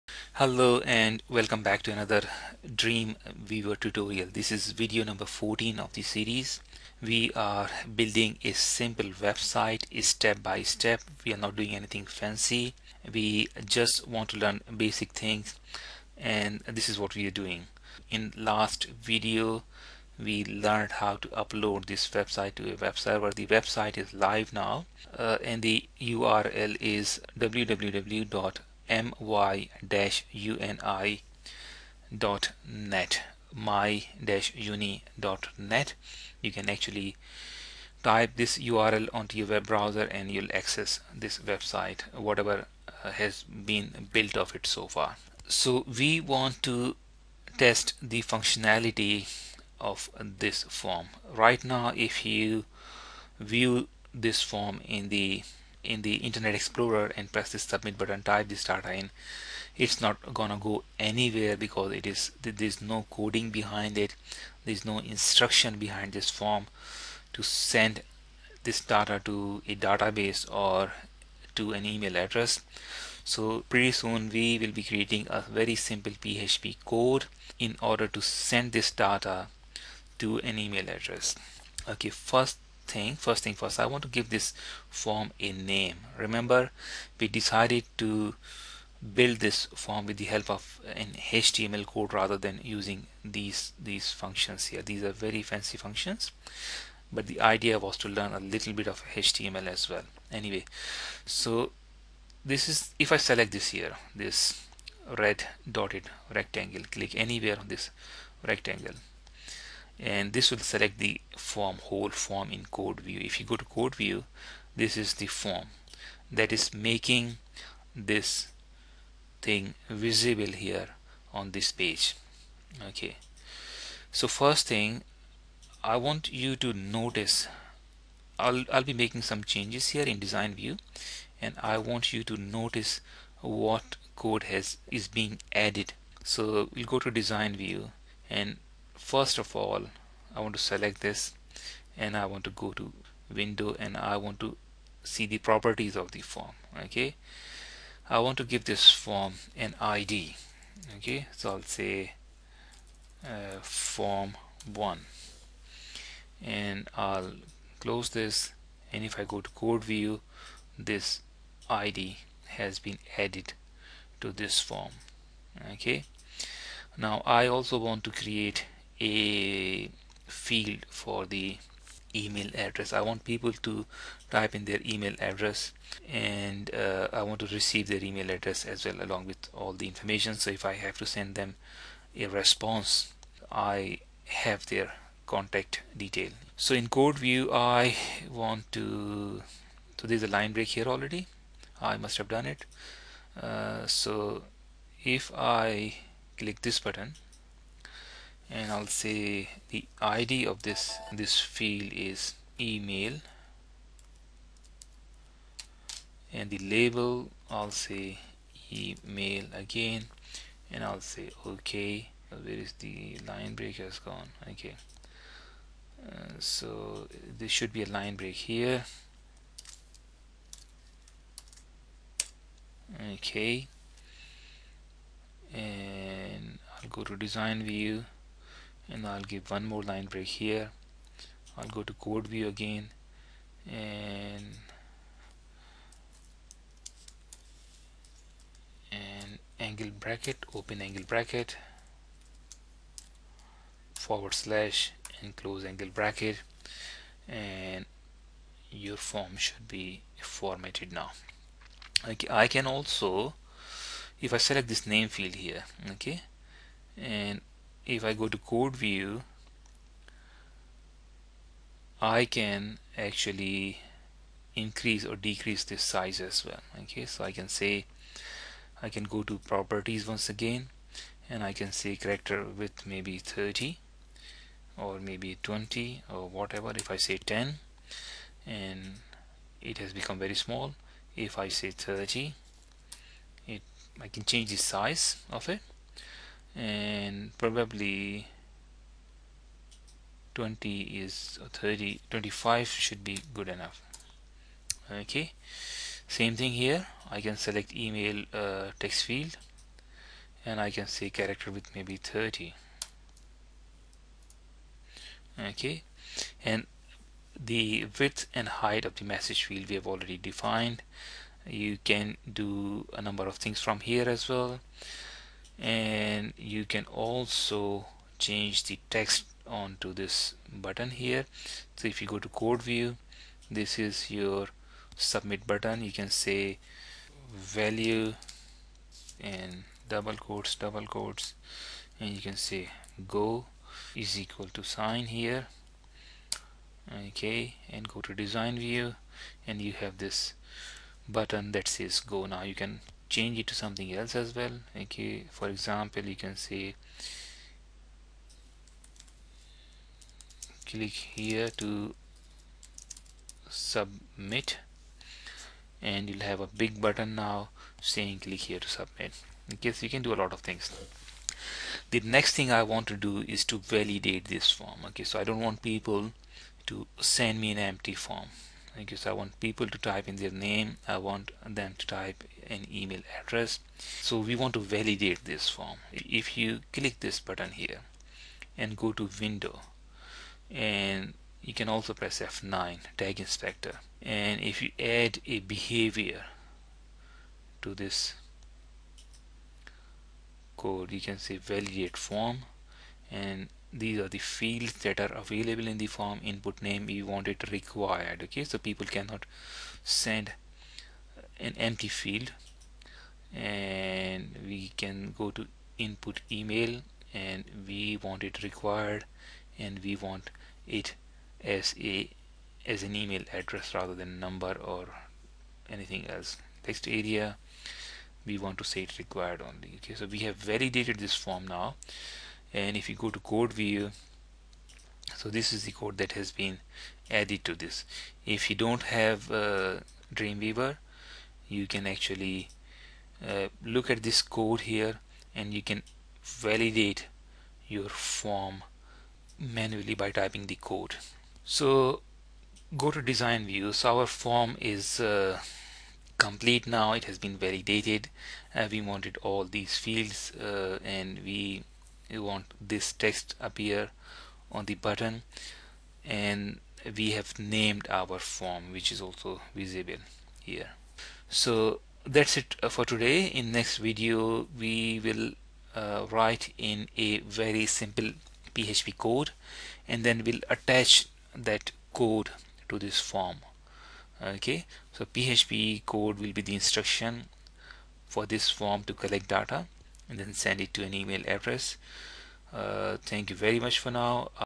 Dreamweaver tutotial 14 - Contact Us form
This is 14th video of the series. Building a website using dreamweaver step by step.